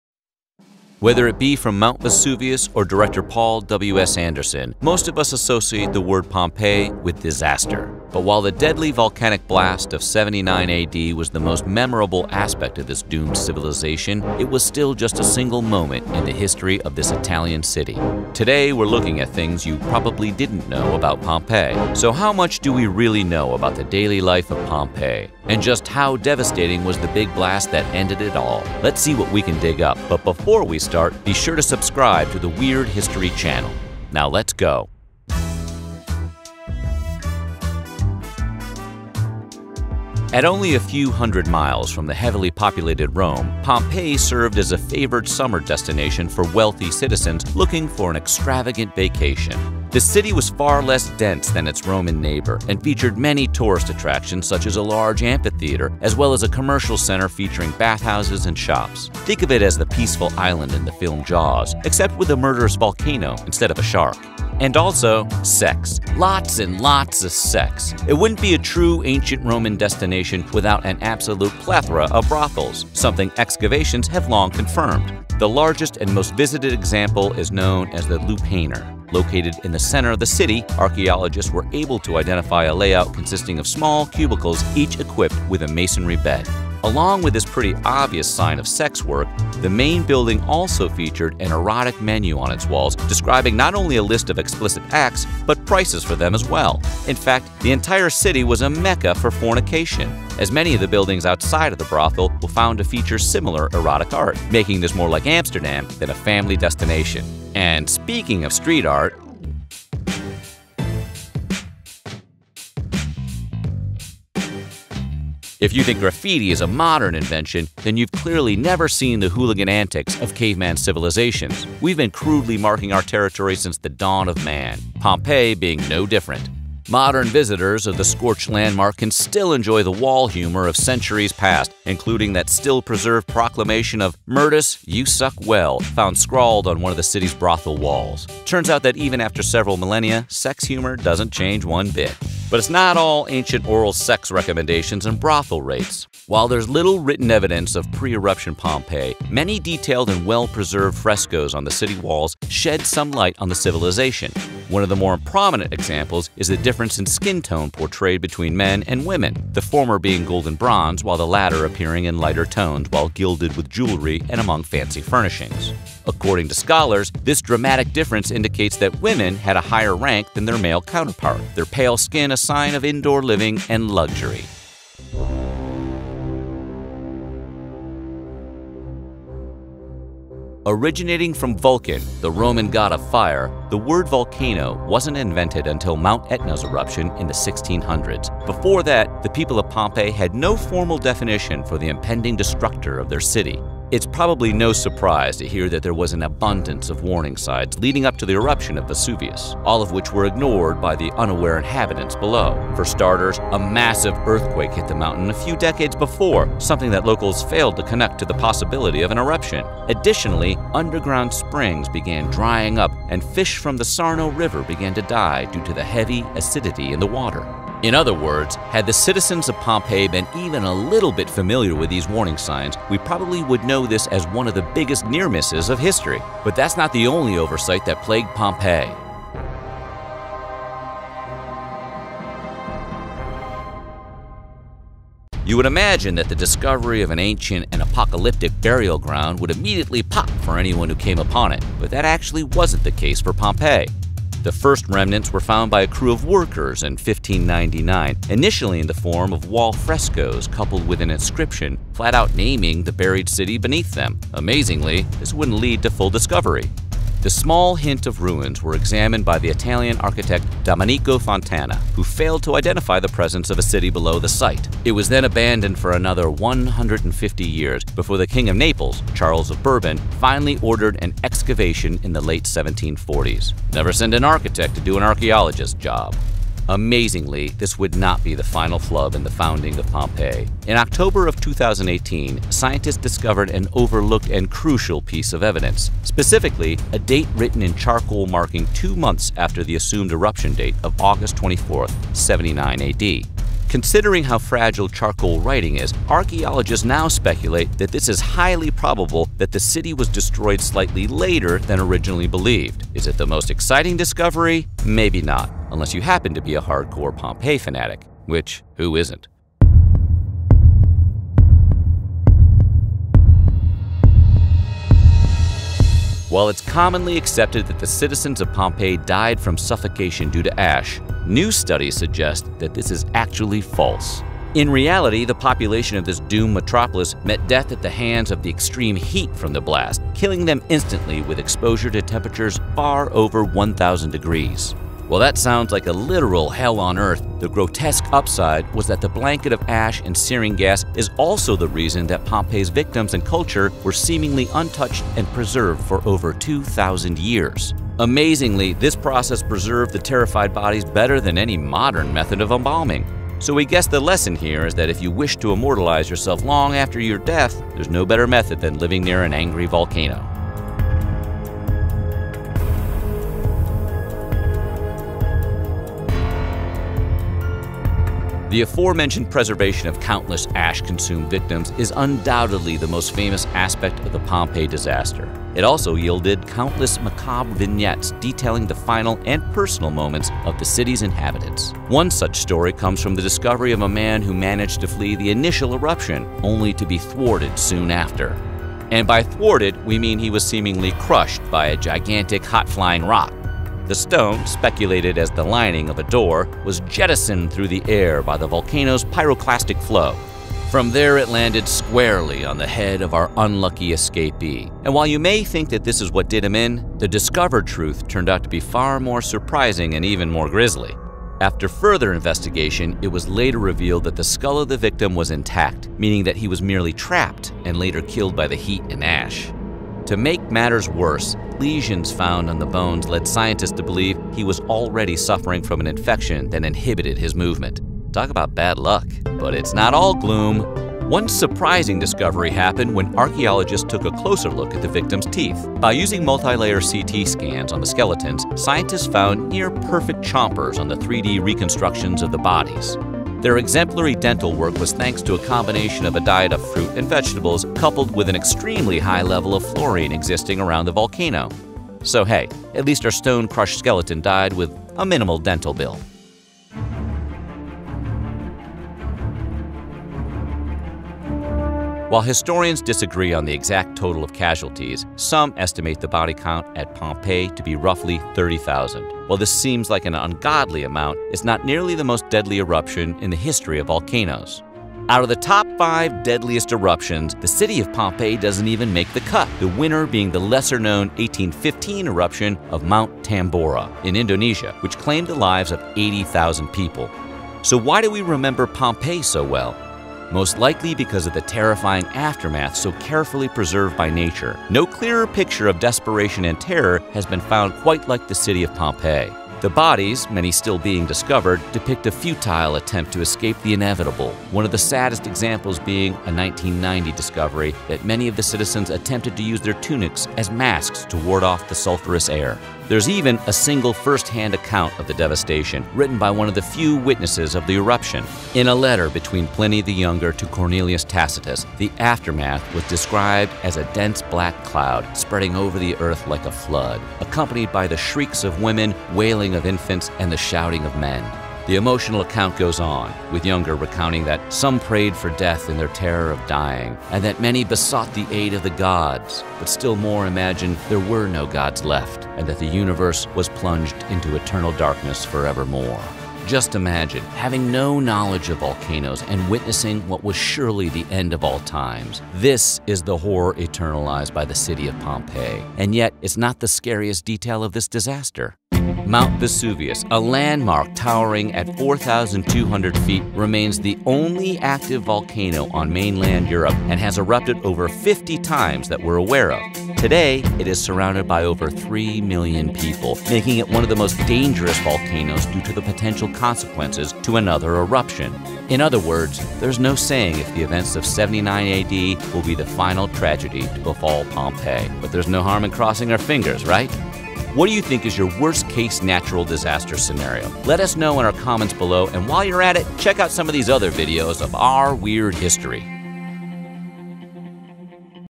Pompeii Facts That Will Blow Your Mind
Mount Vesuvius was responsible for the destruction of the city of Pompeii in 79 AD. Nearly everyone has heard one story or another about arguably the most well-known volcanic eruption in history, but how many of you know what really happened on that fateful day in Pompeii?

This mountain located on the west coast of Italy is one of the most active volcanoes in the world, but the Vesuvius eruption is just one part of the story of the Italian city. How much do you know about daily life before the eruption of Mount Vesuvius? Hopefully, these facts about Pompeii will shed some light on one of the most famous ancient catastrophes in recorded history, as well as the vibrant society that preceded it.